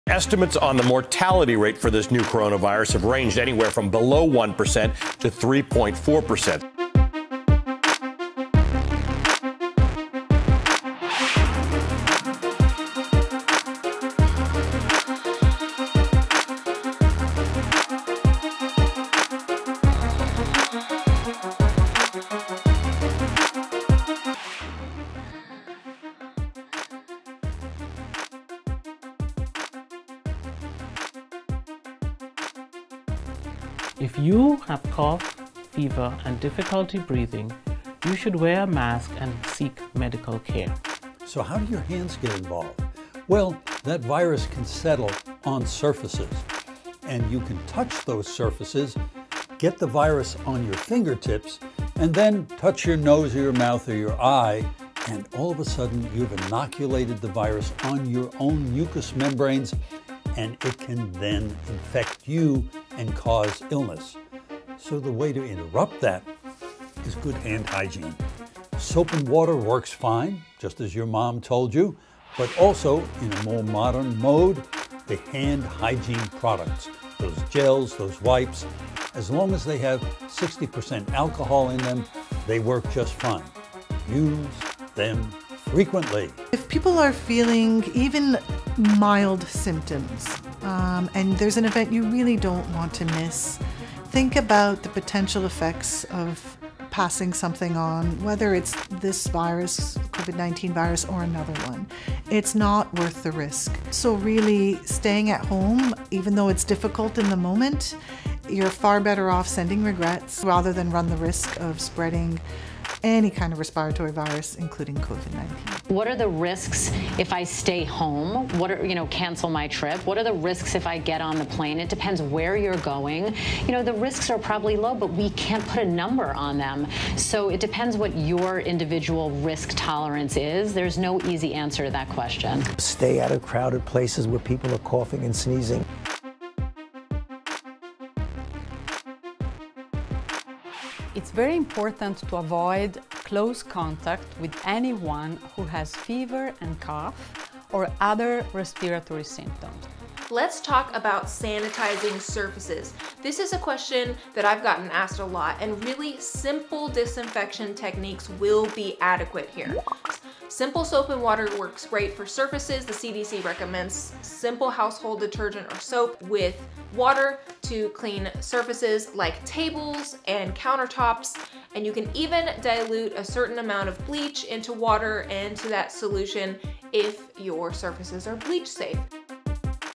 8 ways to prevent Coronavirus
Health and medical professionals share 8 ways to prevent contracting the Coronavirus.